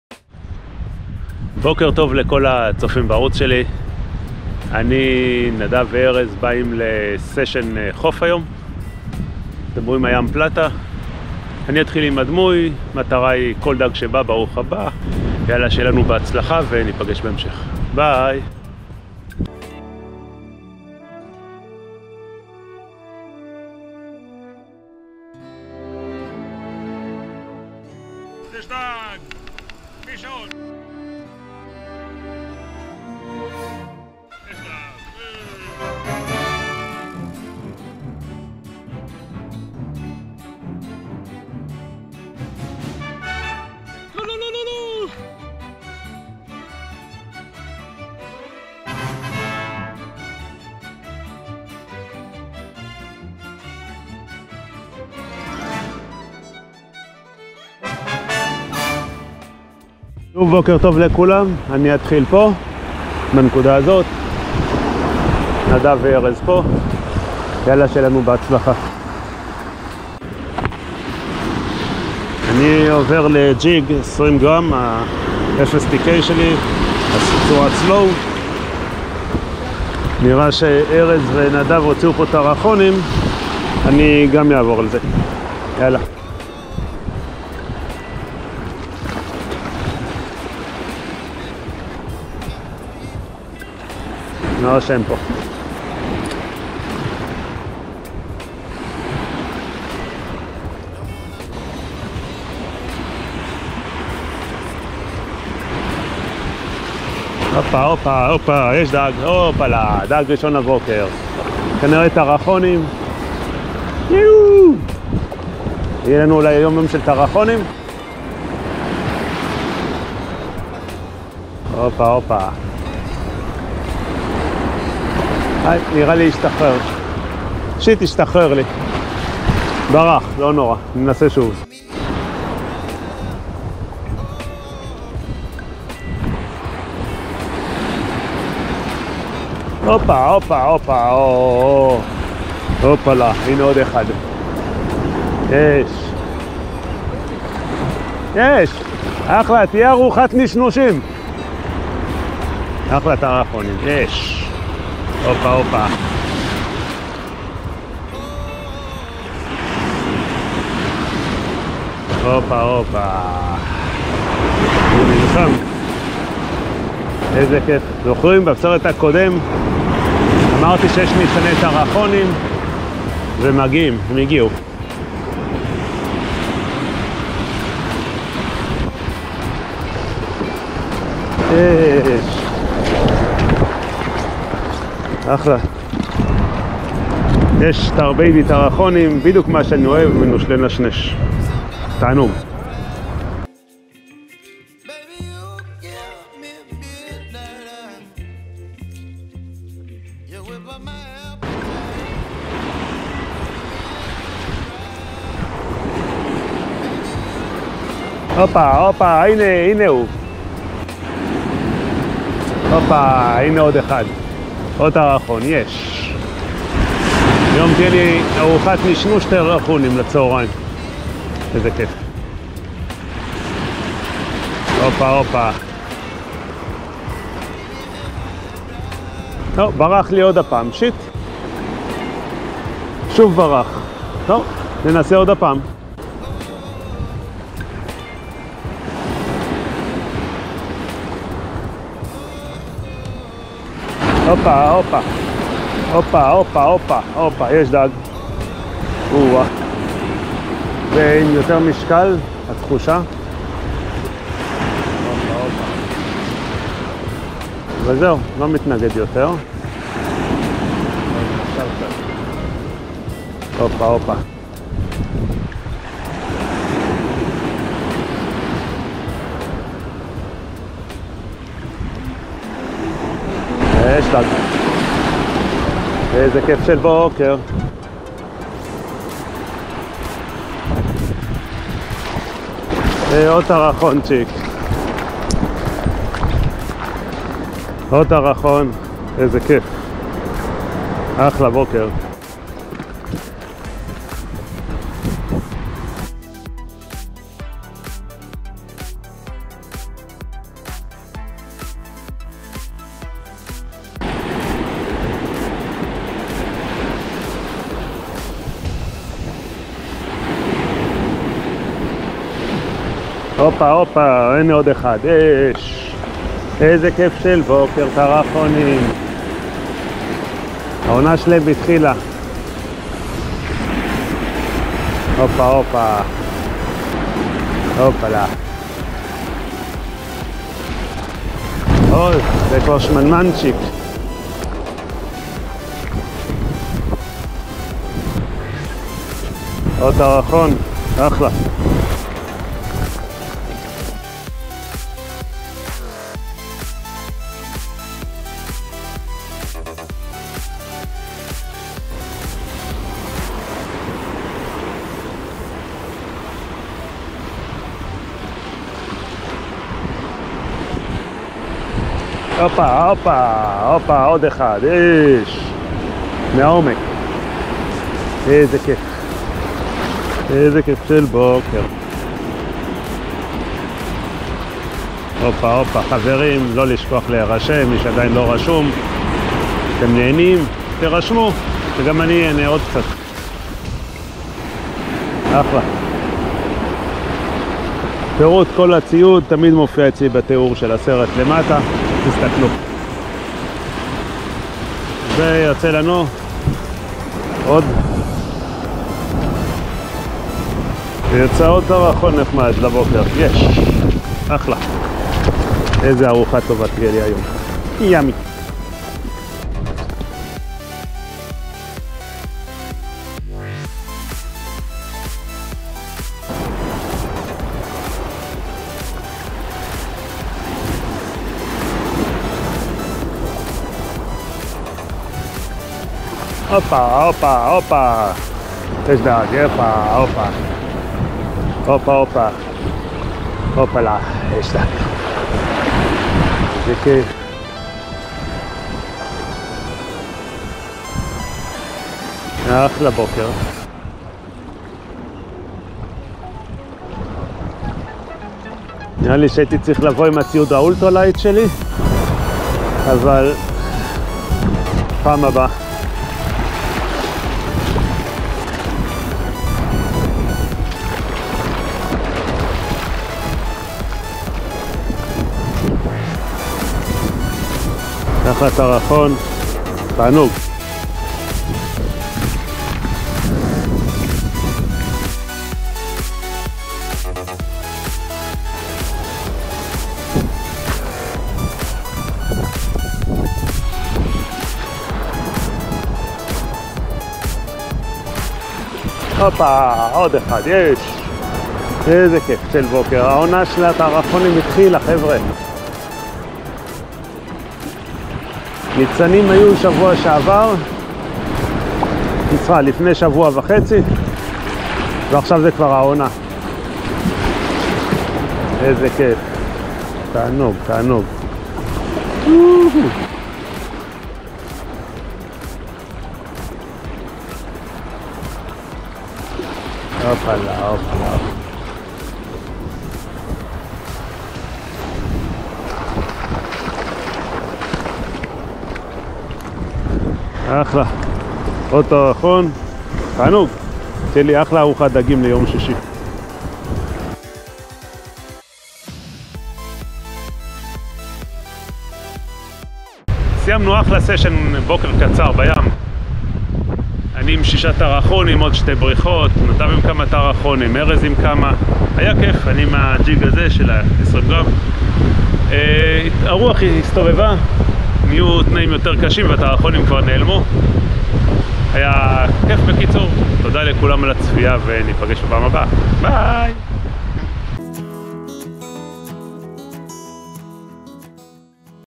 עונת הטרחונים התחילה הבוקר באופן רשמי 🐟👌 Blue Runners in shore jigging season started HE/EN Subs 4K
יום שישי והתחזית היא לים פלטה עם רוח מזרחית ואנחנו (אני, ארז ונדב) יוצאים לחוף במטרה לחפש את הפלמידות/טוניות ו/או כל דג אחר שיגיע.

אנחנו מגיעים לחוף בסביבות 5:20, מתארגנים ומתחילים לדוג.
אני מתחיל עם דמוי ובינתיים ארז ונדב עברו לג'יגים ואחרי כרבע שעה, נדב ננעל על טרחון.

אני ישר מחליף לג'יג הטרחונים שלי של 20 גרם ומנסה את מזלי.
זריקה שניה, כמה הקפצות ויש לי נעילה בעומק ומתחיל פייט מהנה עם טרחון אבל באמצע הדרך הוא משתחרר.
לא נורא, אני מנסה שוב ושוב יש נעילה 💪 ואחרי פייט מהנה יוצא הטרחון הראשון לבוקר הזה.
הוא נכנס לשקית (אני אוהב אותם בגודל הזה😊, הרבה יותר טעימים בטיגון).

עוד כ 5 דקות חולפות ושוב נעילה ושוב הדג משתחרר באמצע הדרך.
לפחות אני יודע שהם פה והם הפכו להיות דג המטרה שלי לבוקר הזה🎯.
עוד כמה דקות ושוב נעילה והפעם הוא לא משתחרר ויוצא עוד טרחון נחמד 🐟.

ככה תוך כחצי שעה, אני מוציא עוד 4 טרחונים, כיף לראות שהם כאן והעונה התחילה באופן רשמי.

בפעם הבאה אני אגיע כבר עם ציוד האולטרה לייט שלי להנות יותר.

בסביבות 6:30, הרוח השתנתה לדרומית והטרחונים פשוט נעלמו ולא שיתפו יותר פעולה.
עוד כחצי שעה של סריקת שטח, הרוח מתחזקת ואנחנו מחליטים לפרוש מוקדם.

תהנו מהסרט ולא לשכוח להרשם לערוץ 🤝

הציוד שהשתמשתי בו:
לידר 35LB YGK
דמוי:
ג'יג 20 גרם FSTK